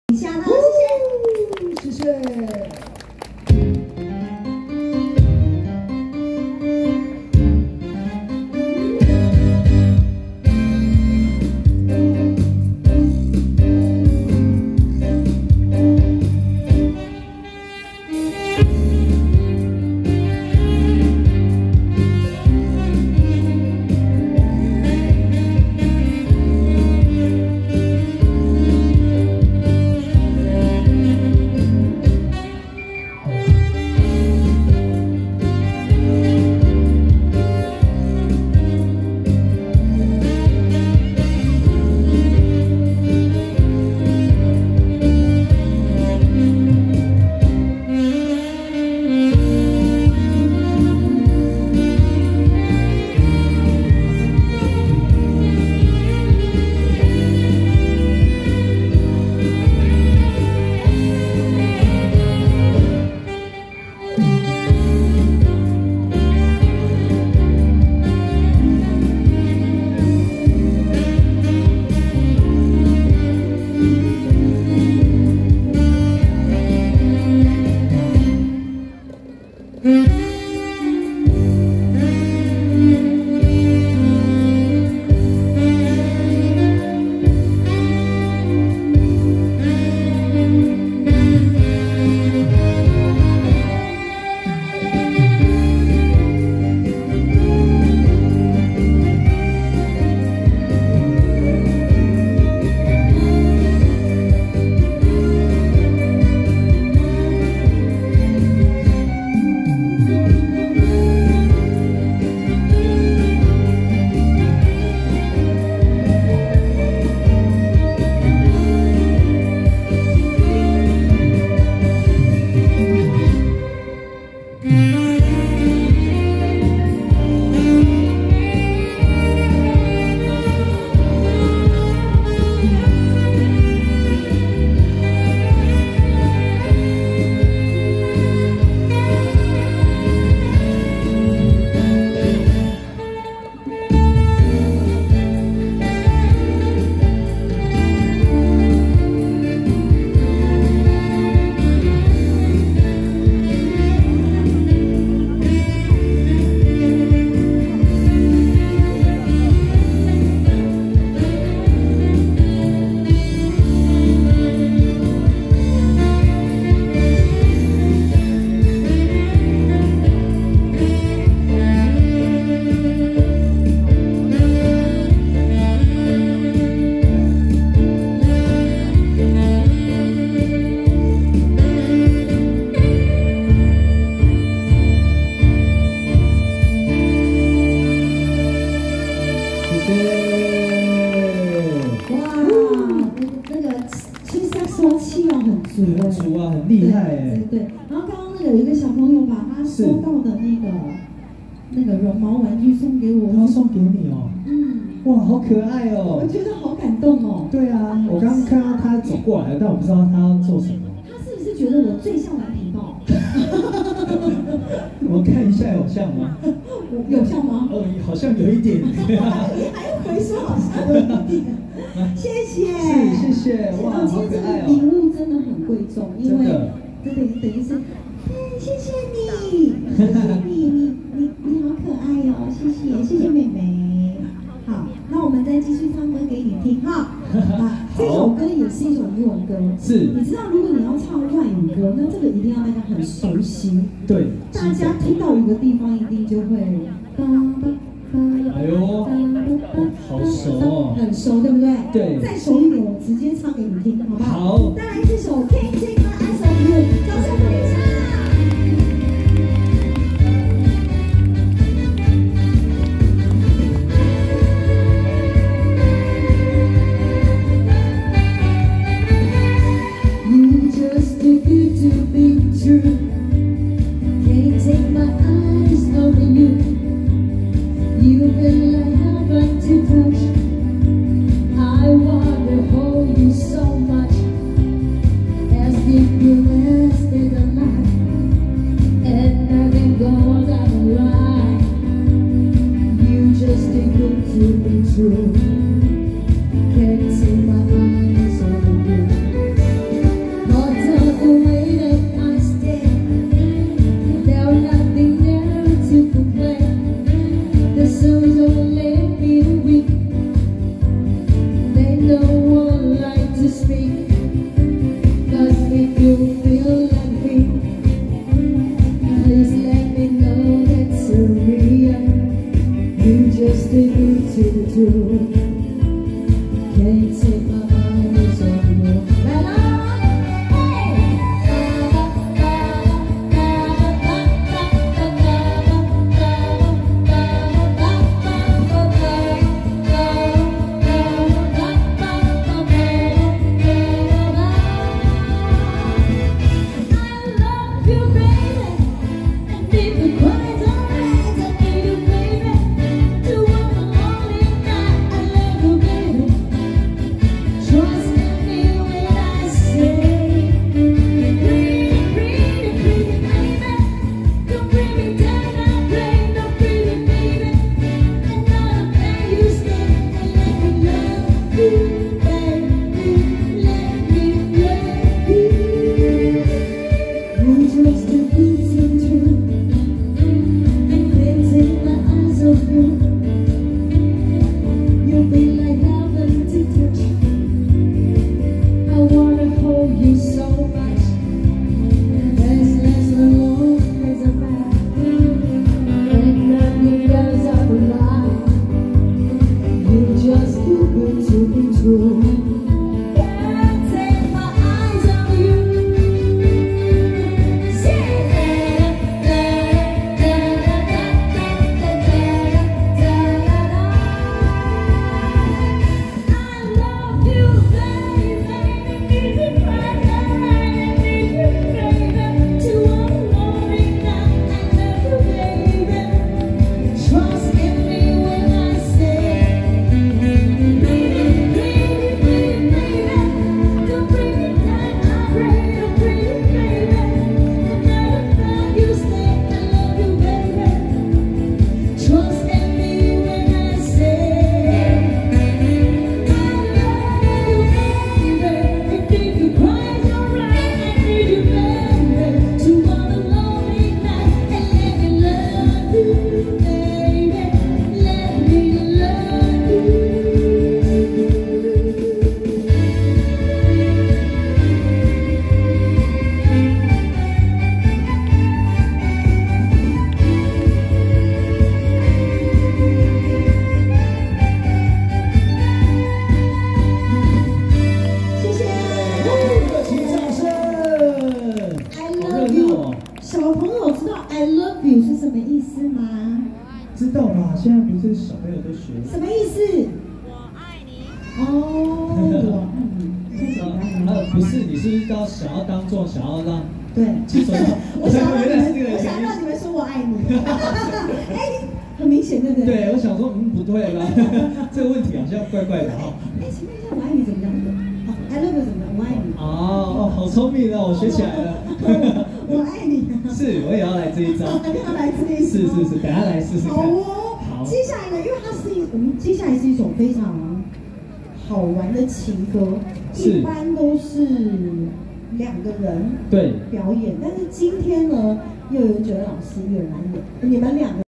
2022議起愛相隨 音樂會(3)
2022議起愛相隨 音樂會(3)

2022/10/07 20:13 攝於安平區永華路二段旁的#台南市議會親水公園 。
表演樂團為#琴硯樂團。